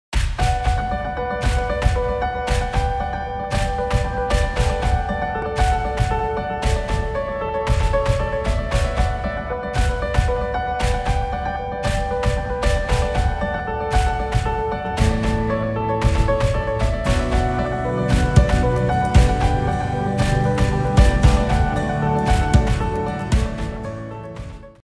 SellEton Heavy Duty Z-CR WLSSP Wireless Crane Scales
SellEton's Z-CR-WLSSP wireless Crane Scales are great for industrial applications. you can easily operate the scale wirelessly up to 300 ft away. It is water proof / IP67 & ready to be used out-doors.